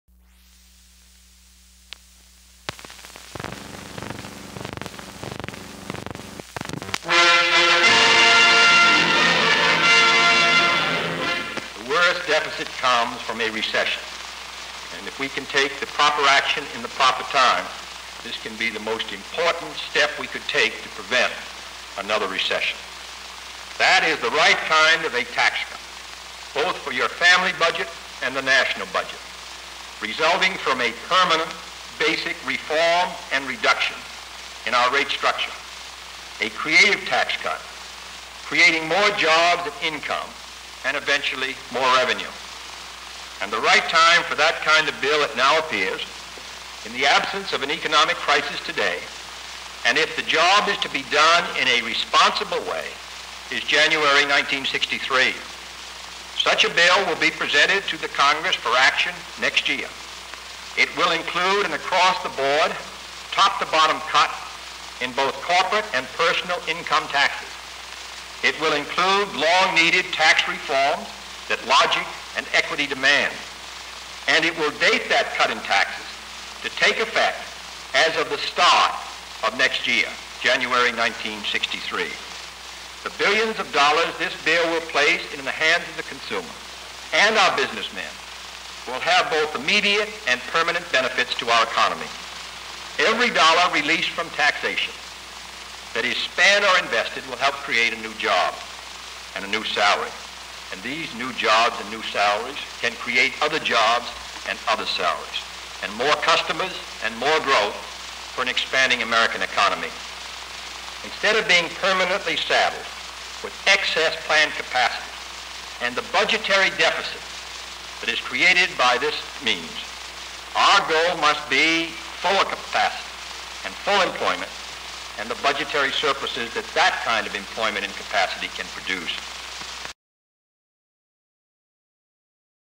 HD Stock Footage Kennedy Proposes Tax Cut to the Nation
Newsreel 1962, August 13
President John F. Kennedy Proposes Tax Cut To Nation
In an address to the nation, the President outlines the status quo of the economy of the United States, and his hopes for a cut in corporate and personal income taxes early in I963.